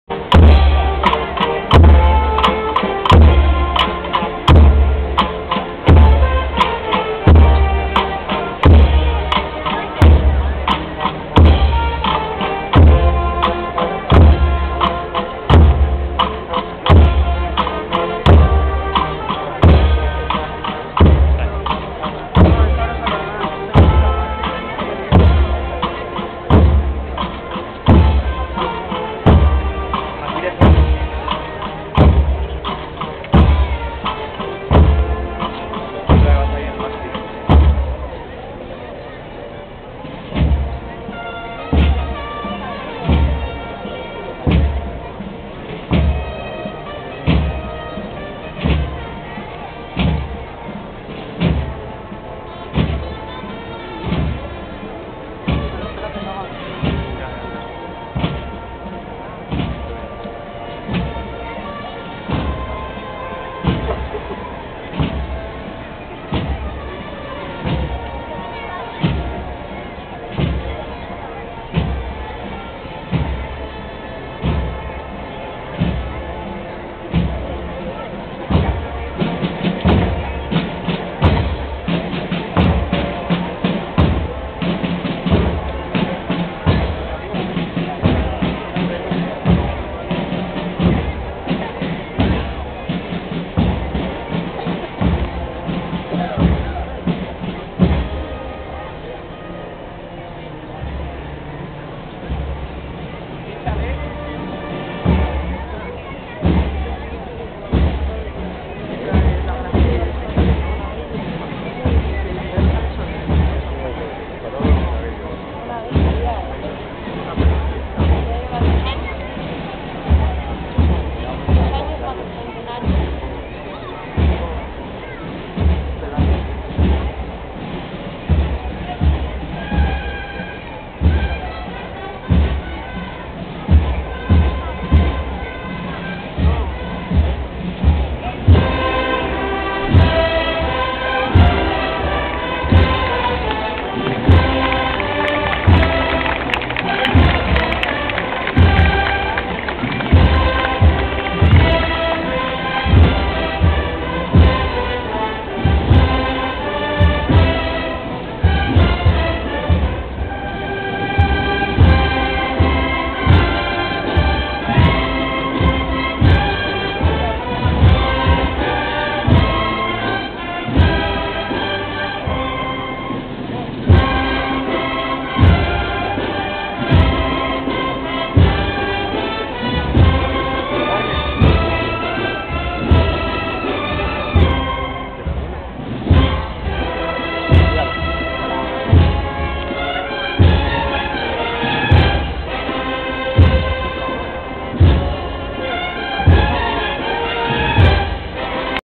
Salida del Nazareno Lunes de Pasiòn en Leòn 2011.
Sale de Santa Nonia el Titular de la Cofradia del Dulce Nombre de Jesus LEÒN.
El Nazareno.